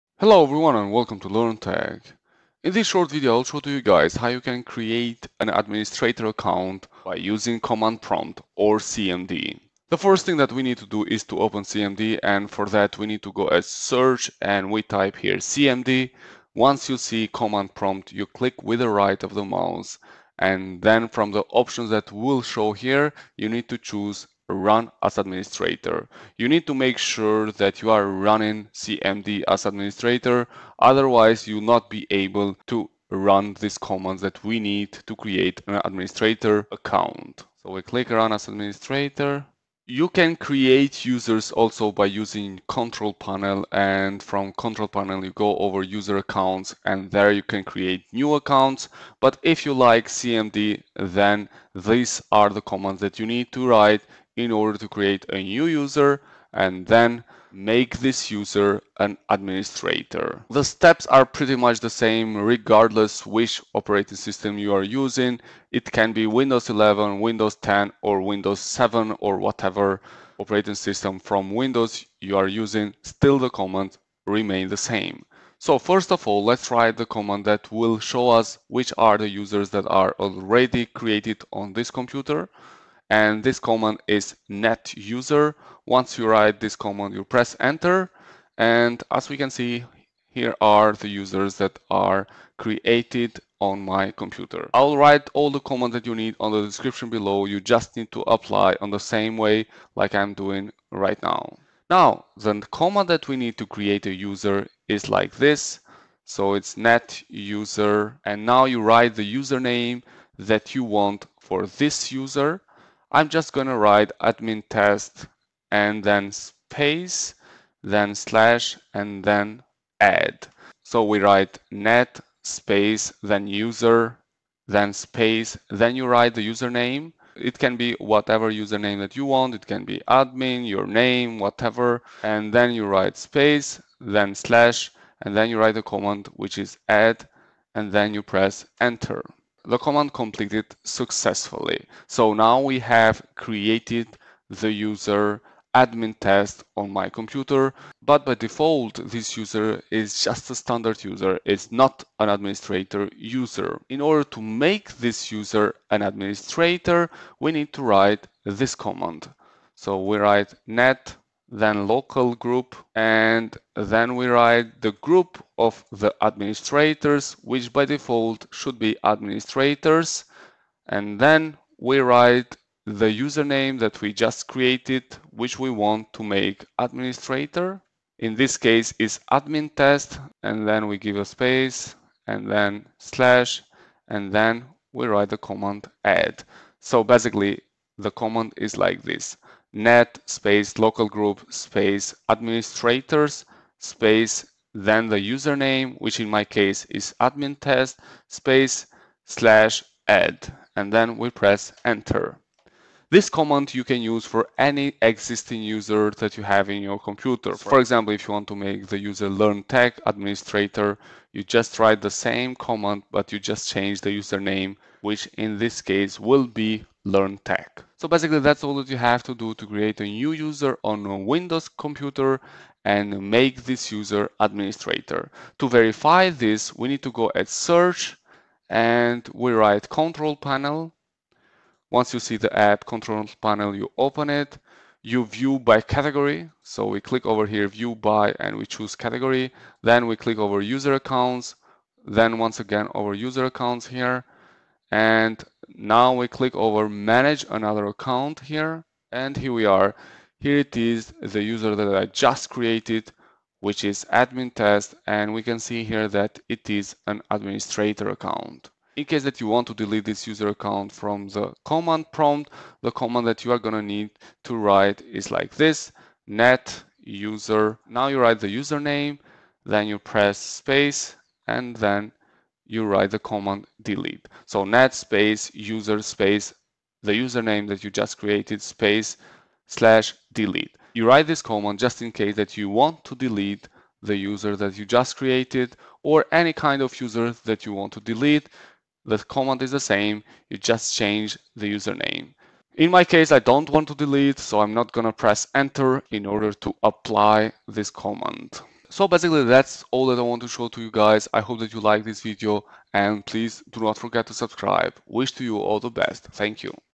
Create a New Administrator Using CMD in Windows 10 & Windows 11
The commands that needs to be used to create an administrator account:
net user
net user admintest /add
net localgroup administrators admintest /add
net user admintest /delete